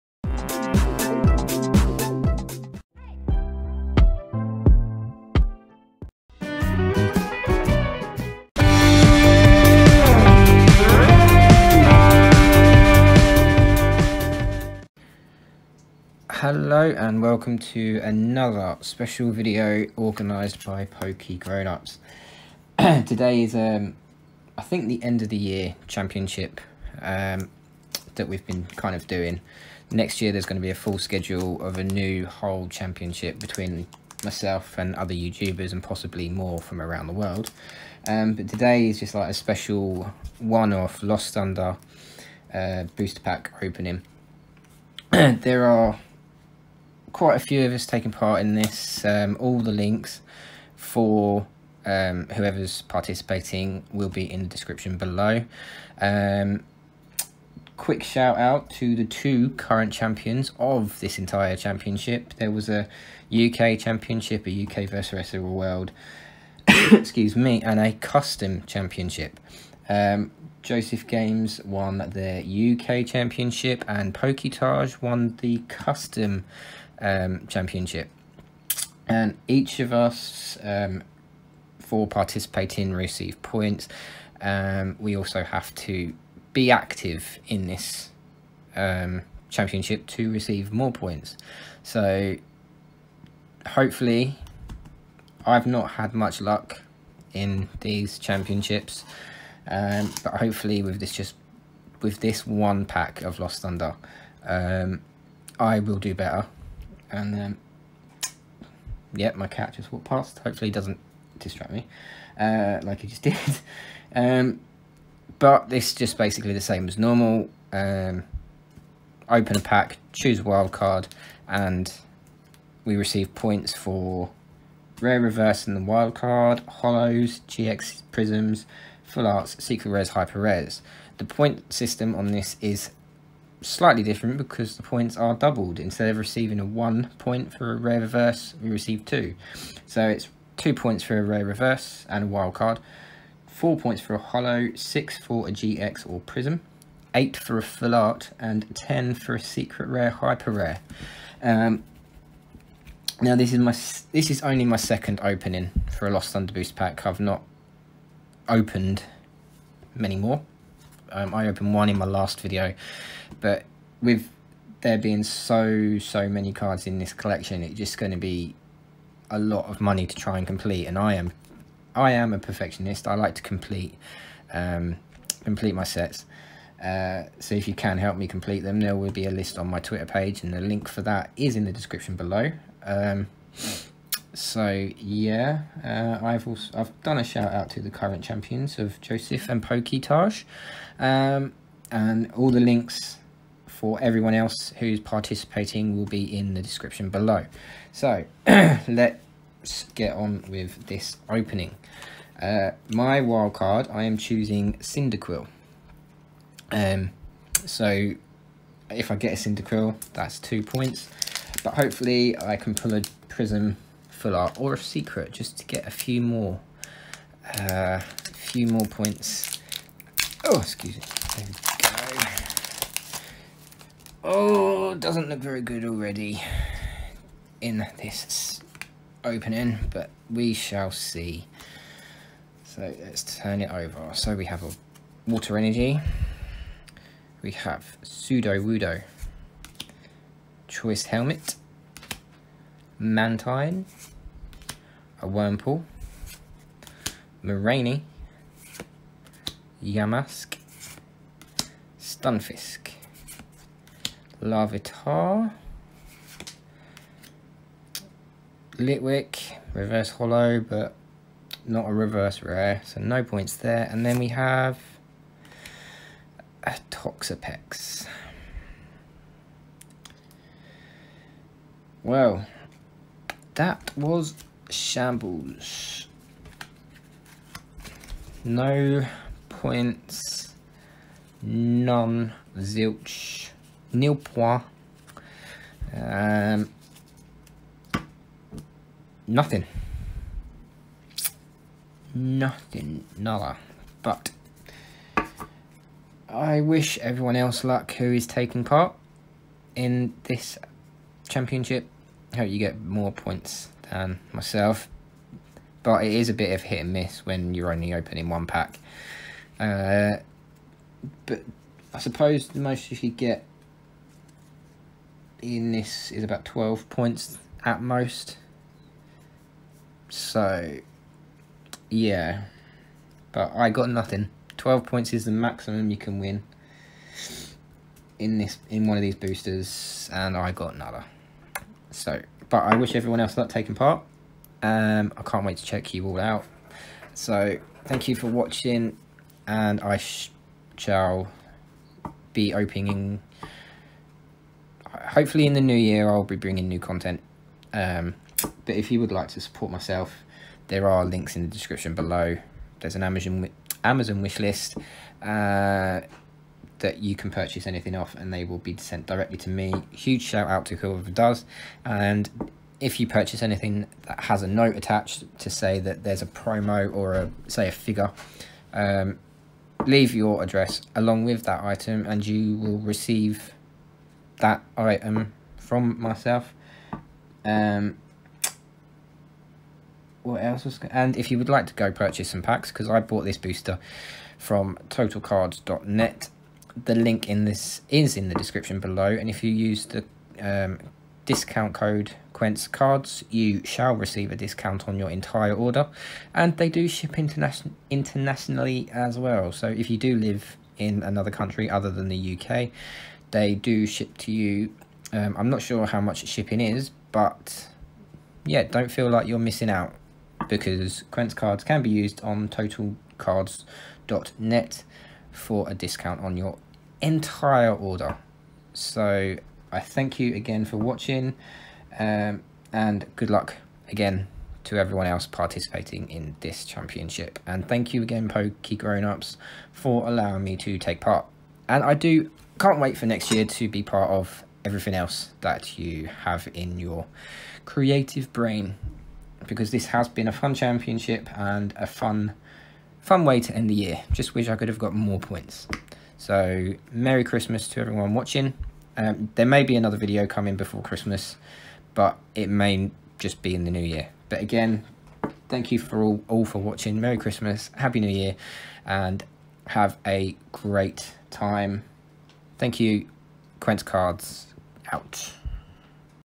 Elite Championship Single Lost Thunder Booster Pack Opening
and use the 'QuentsCards' at checkout to receive a discount

Amazon Wishlist

Now on Instagram: QuentsCards

Today is a very special video, part of PokeGrownUps Pack Battle Championship.

They again, have done an AMAZING job at organising this, and every other battle I, and everyone else has participated in.